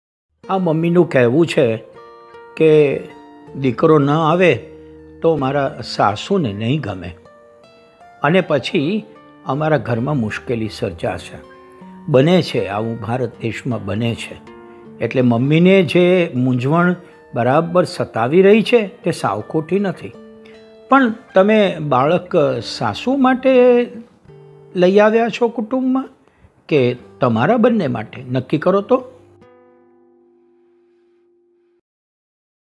ઈશ્વરના ઓટોગ્રાફ - ૩૧ : નકકી કરો તો...
(ઈશ્વરાંશનું સ્વાગત - સ્વીકાર - સત્કાર)
કેવળ ત્રીસ સેકન્ડમાં બાળ હૂંફ માટેની અણમોલ ટિપ્સ

Bhadrayu Vachhrajani